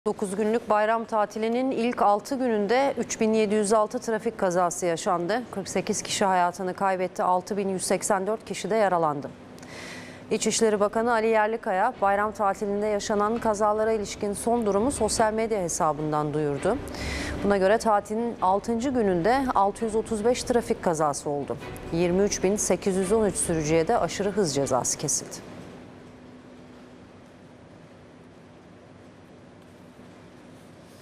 Bayram Tatilinin Altıncı Gününde Acı Bilanço: 48 Ölü, 6148 Yaralı | NTV
Bakan Yerlikaya Bayram Tatilinin Altıncı Gününde Acı Bilançoyu Açıkladı!